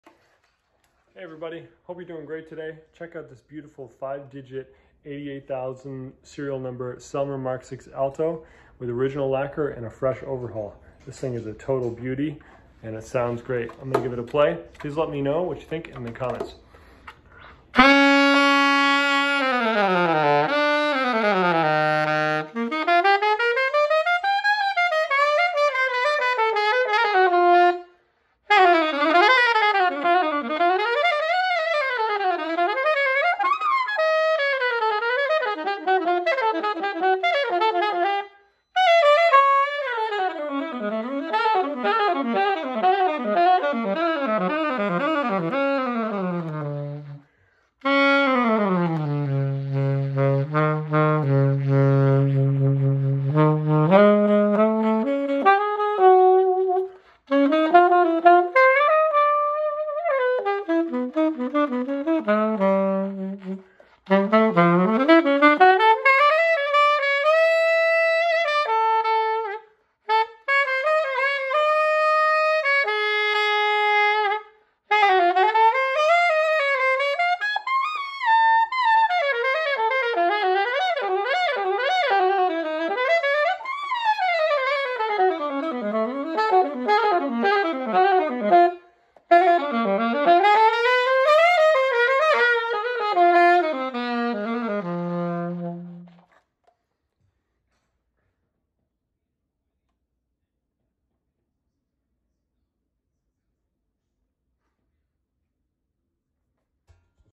Listen to this GORGEOUS 88xxx Selmer Mark VI Alto Saxophone fully overhauled!
Absolutely Beautiful!!! And plays SO killer! 🔥🔥🔥

Just arrived 5 Digit Original Lacquer Mark VI Alto! 88xxx

Check out this Stunning 5 Digit Selmer Mark VI Alto Saxophone that just arrived to our shop for sale! 88xxx serial number places the year of production to approximately 1960 and makes it one of the early long bow versions of the Mark VI Altos. When this saxophone arrived to our shop it was in its original case had one owner and had all of the original pads still in it. We have since given it a full professional level overhaul in our sax shop and it is playing like brand new. Even the original lacquer is nearly 100% intact on this horn and it is stunningly beautiful! There is no neck pull down at all and the neck has the original matching serial number stampings. There are no dents or dings in this incredible Mark VI Alto just a small amount of superficial scratching in the lacquer. Not even any Dents in the Bottom Bow Rooster Tail which is incredibly common. Just look at how crisp and Beautiful the American Engravings are on the Bell! 😯 wow….